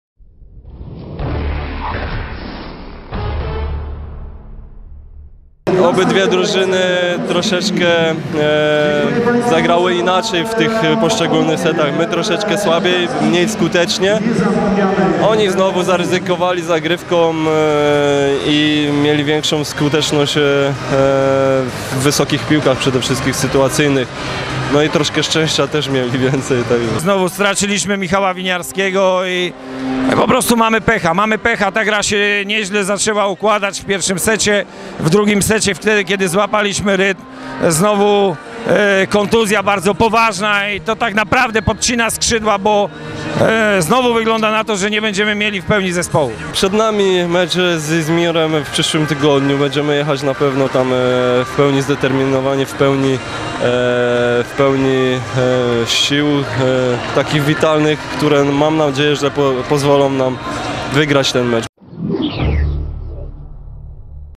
Delecta_PGESkra
Zobacz materiał telewizji SKRA.TV z meczu Delecta Bydgoszcz - PGE Skra Bełchatów 3:1